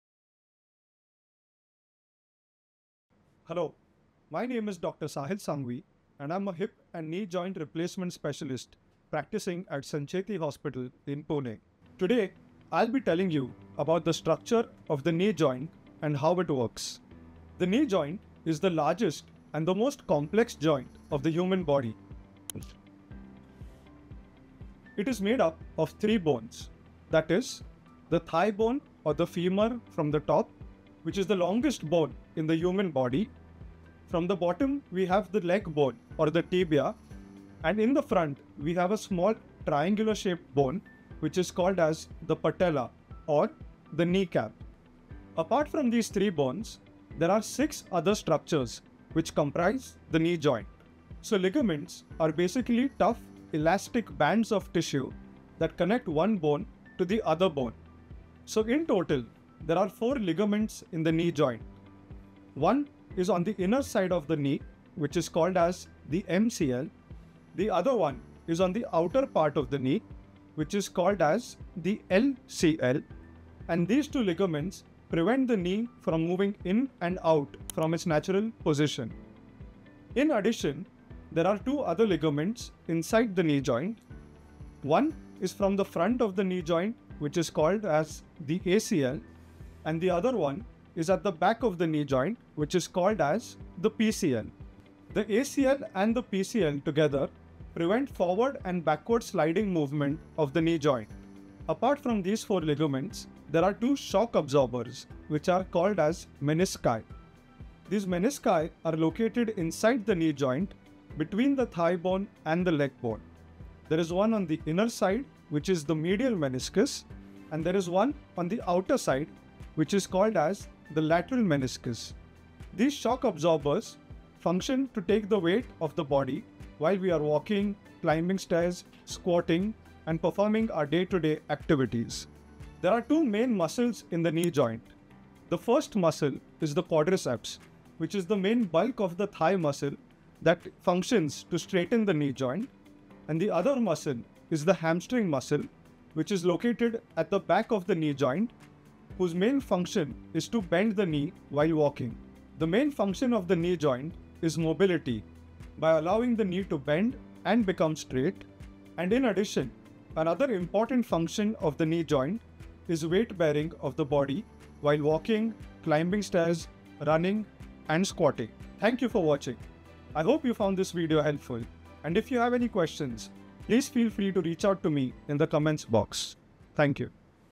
Knee Joint Structure & Function Explained | How the Knee Works (Bones, Ligaments & Movement)
Structure of the Knee Joint & How It Works | Explained by Dr. Sahil Sanghavi 🦵

In this detailed video, Dr. Sahil Sanghavi breaks down the anatomy and biomechanics of the knee joint, one of the most complex and vital joints in the human body. You'll learn about the bones, ligaments, cartilage, tendons, and muscles that make up the knee and how they work together to allow movement, provide stability, and support your body weight.

Whether you're a medical student, physiotherapist, athlete, or just curious about your body, this video is designed to be informative, visual, and easy to understand.

👉 Topics covered:

Bones of the knee (femur, tibia, patella)

Ligaments (ACL, PCL, MCL, LCL)

Meniscus function

Muscles acting on the knee